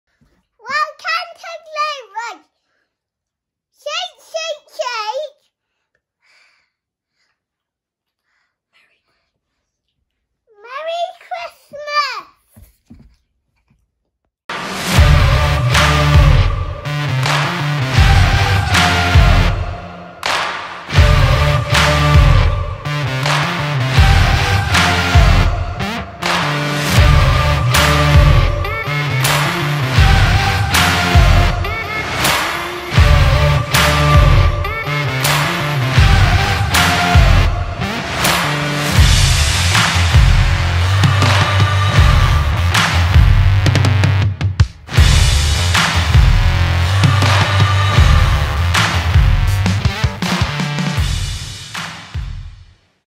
glutewod with Lillie Willow Peaches
Annnnnd glutewod drops!

20 kB swings
20 jump lunges
20 glute bridges
20 banded jump squats
3 rounds for the heart rate and the burn!